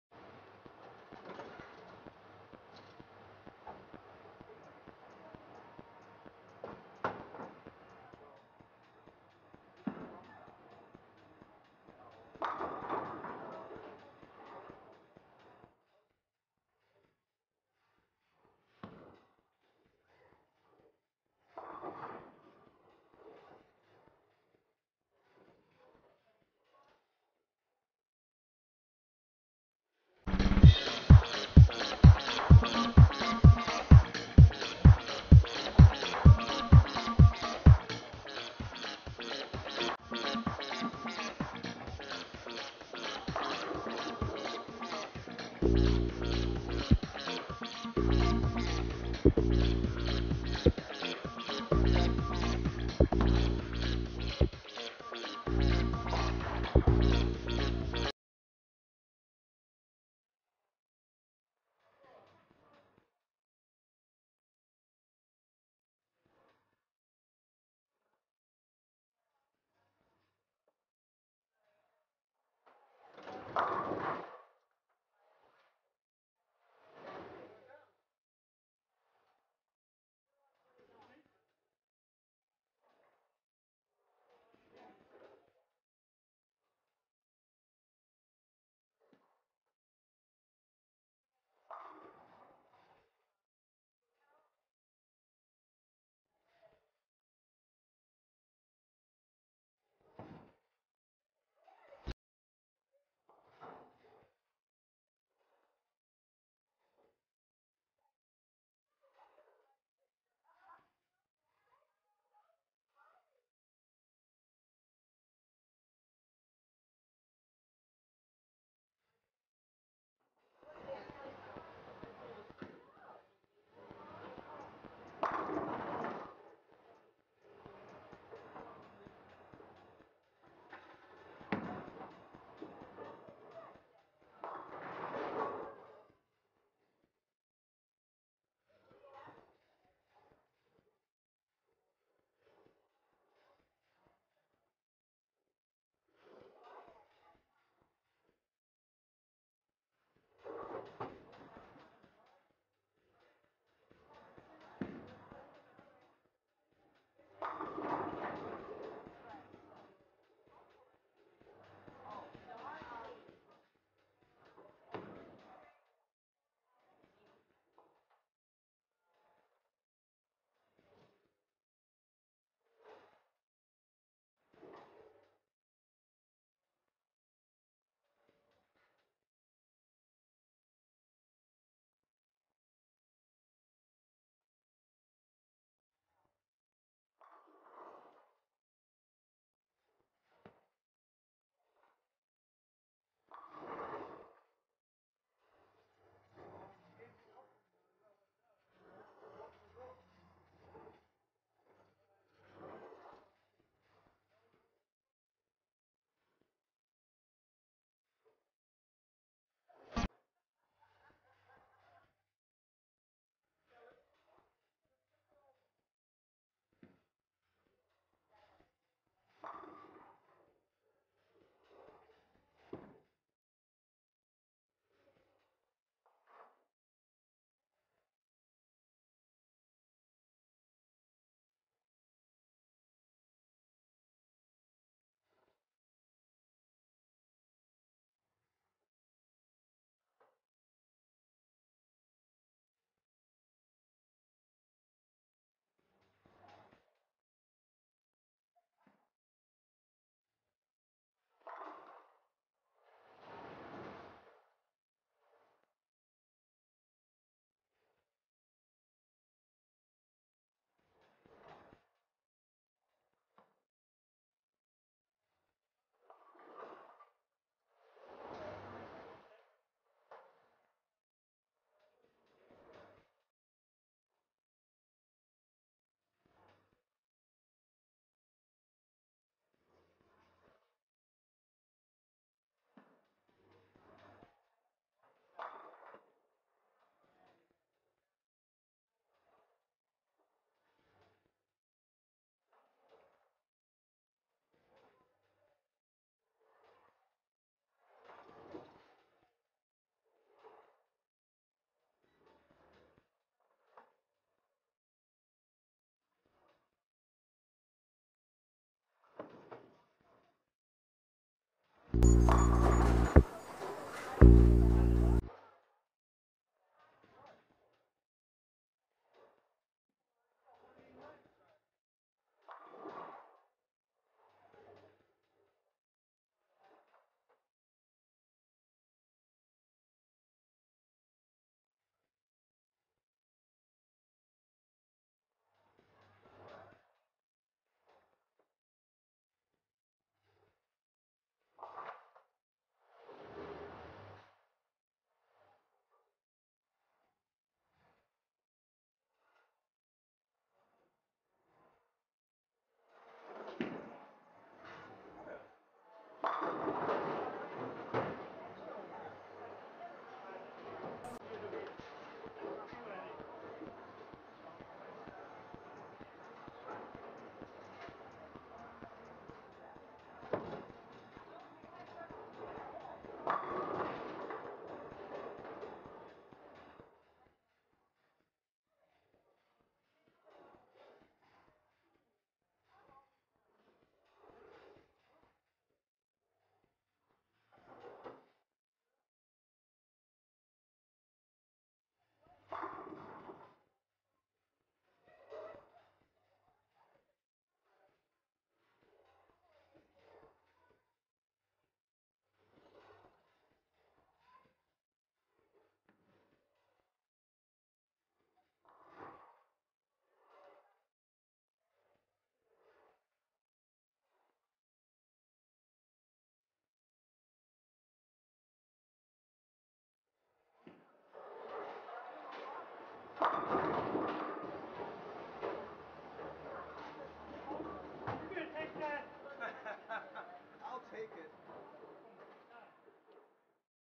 Copy of Roto Grip Disturbed Again
More practice with Roto Grip Disturbed the approach was sticky. Threw my balance off a little not that it needed help